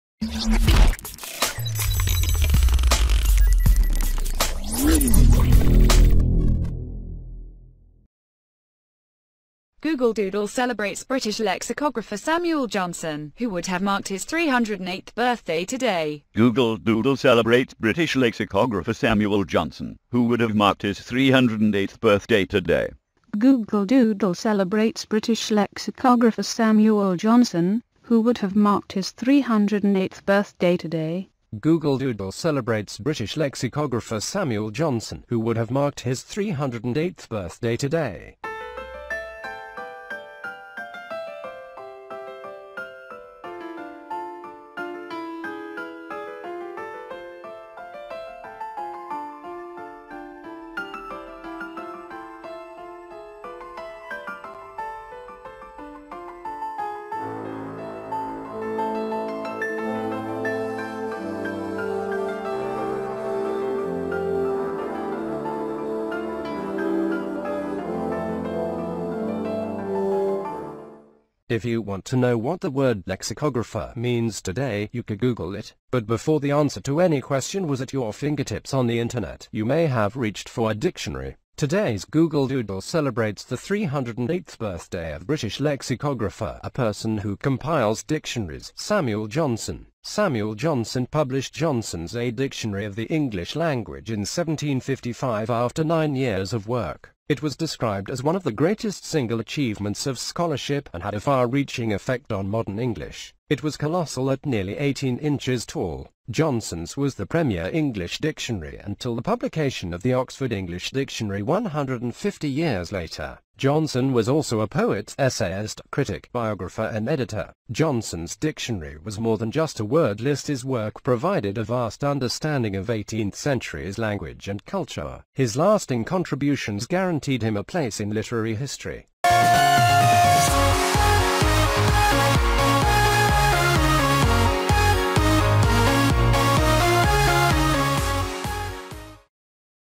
If you want to know what the word 'lexicographer' means today, you could Google it .
Who was Samuel Johnson? The story behind the man who made the main English lexicon

Google Doodle observes British word specialist Samuel Johnson, who might have denoted his 308th birthday celebration today

On the off chance that you need to comprehend what "word specialist" implies today, you could Google it .

In any case, before the response to any inquiry was readily available on the web, you may have gone after a word reference.

The present Google Doodle commends the 308th birthday celebration of British etymologist – a man who accumulates lexicons – Samuel Johnson .

Samuel Johnson distributed Johnson's: A Dictionary of the English Language in 1755 following 9 years of work.

It was depicted as "one of the best single accomplishments of grant," and had an expansive impact on current English.

It was "monster" at almost 18 inches tall! Johnson's was the head English word reference until the production of the Oxford English Dictionary 150 years after the fact.

Johnson was likewise a writer, writer, faultfinder, biographer and editorial manager. Johnson's lexicon was something beyond a word list: his work gave an immense comprehension of eighteenth century's dialect and culture.

His enduring commitments promised him a place in abstract history.